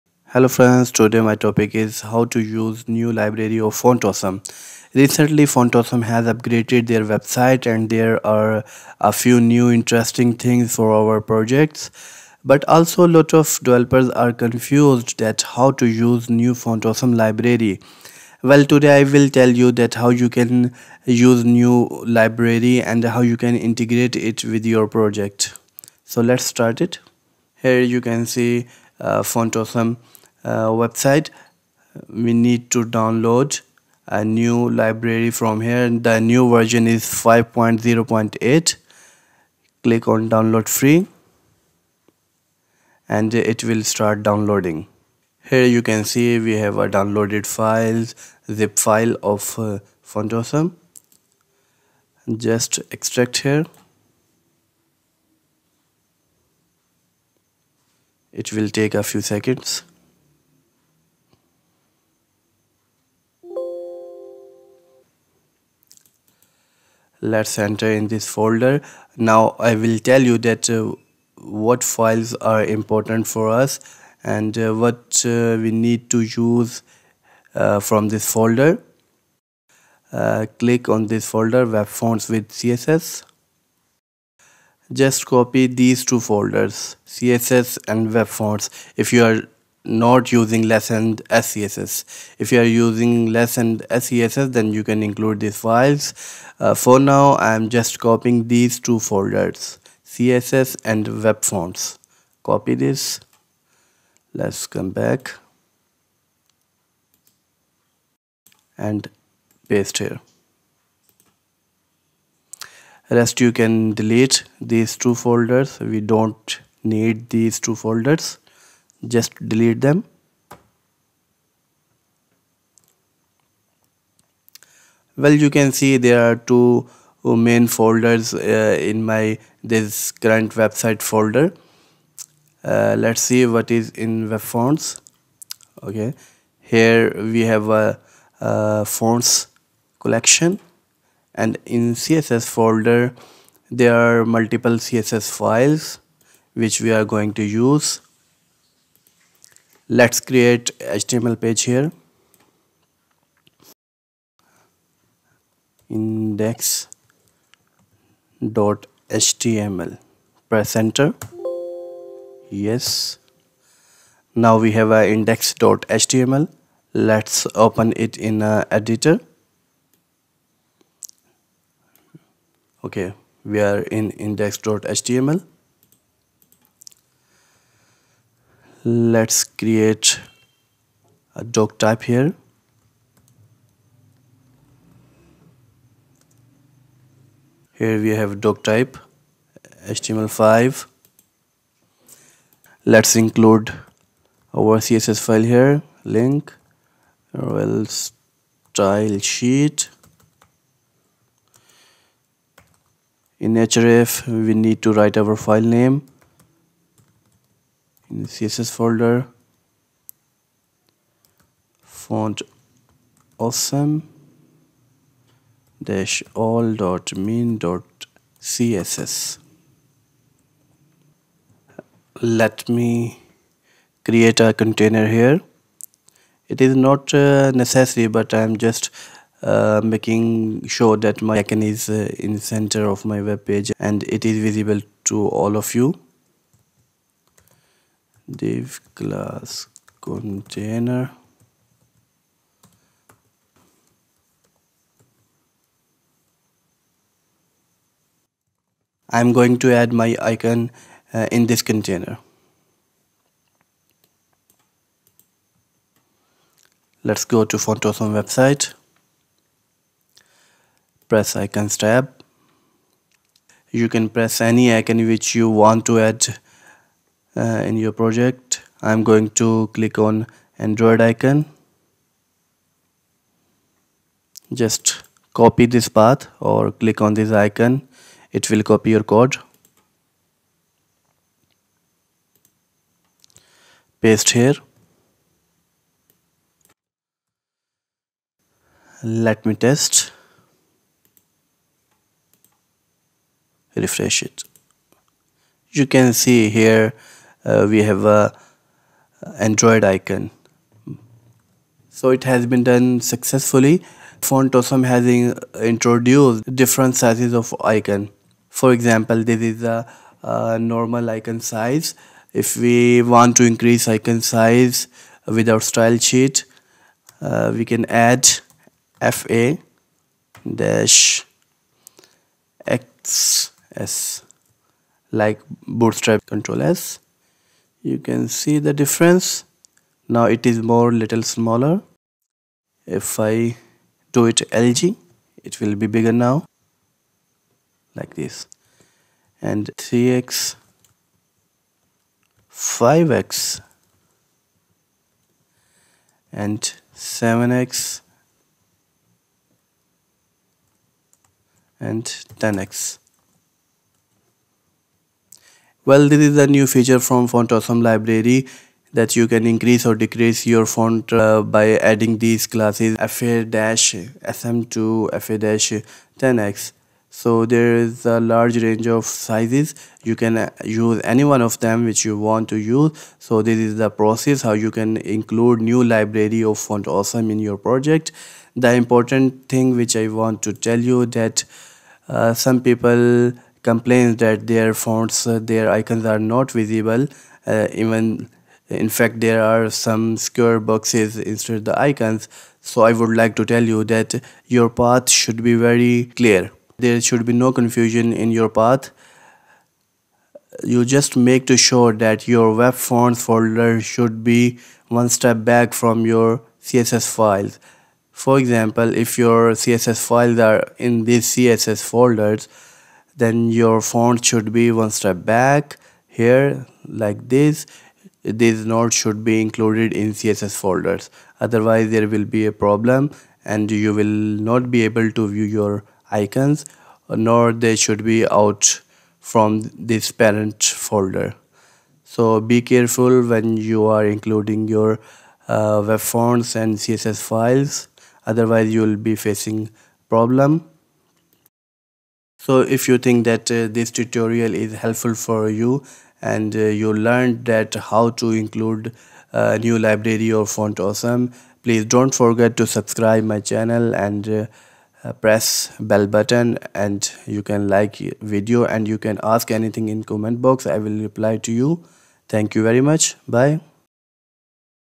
How to use Font Awesome in HTML CSS | Latest Version
How to use new version of font awesome in project. Font Awesome latest version 5.0.8 and new CSS files. Use font awesome library in HTML CSS. Font Awesome new classes xs, sm, lg, 2x, 3x, 5x, 7x, 10x.